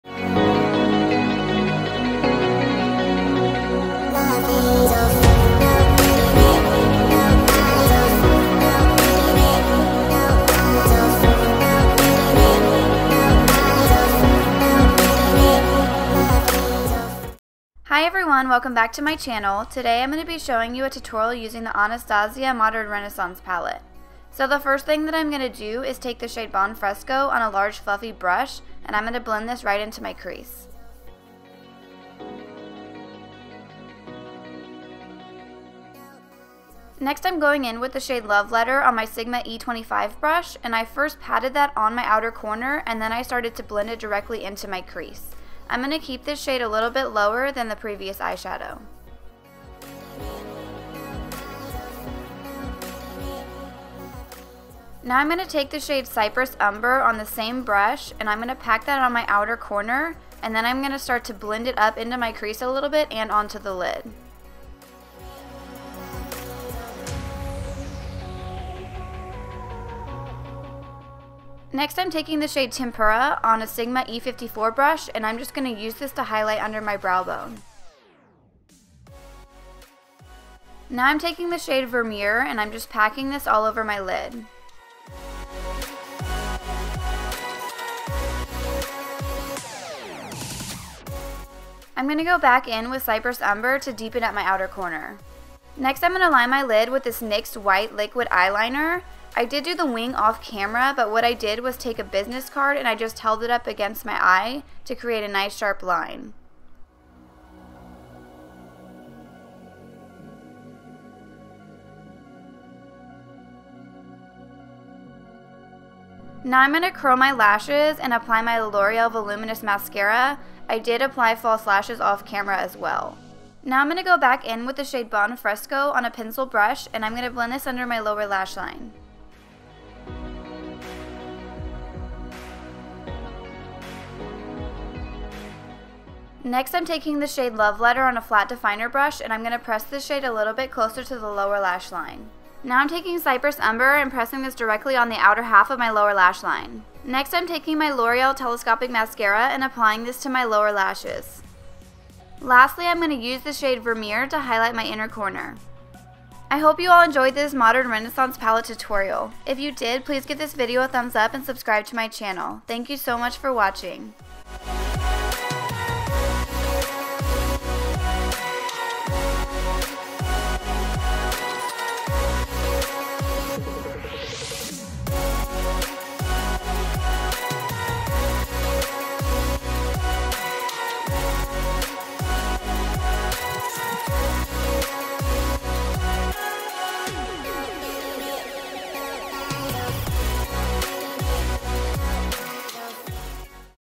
ABH Modern Renaissance Palette Tutorial
Hi everyone! I hope you all enjoy this ABH Modern Renaissance Palette Tutorial with White Winged Eyeliner!

PRODUCTS:
Anastasia Beverly Hills Modern Renaissance Palette
NYX White Liquid Eyeliner
Loreal Voluminous Mascara
Ardell Studio Effects 110 Lashes
House of Lashes Lash Glue
Loreal Telescopic Mascara

BRUSHES:
IT Brushes for Ulta - Airbrush Blending Crease Brush #105
Sigma E25 Blending Brush
Sigma E54 Medium Sweeper
Sigma E15 Flat Definer Brush
Real Techniques Eyeliner Brush

EQUIPMENT:
Canon EOS Rebel SL1 DSLR Camera
Canon EF 50mm f/1.8 STM Standard Lens
Soft Box Lighting Kit
Backdrop Stand
Sequin Backdrops
Paper Backdrops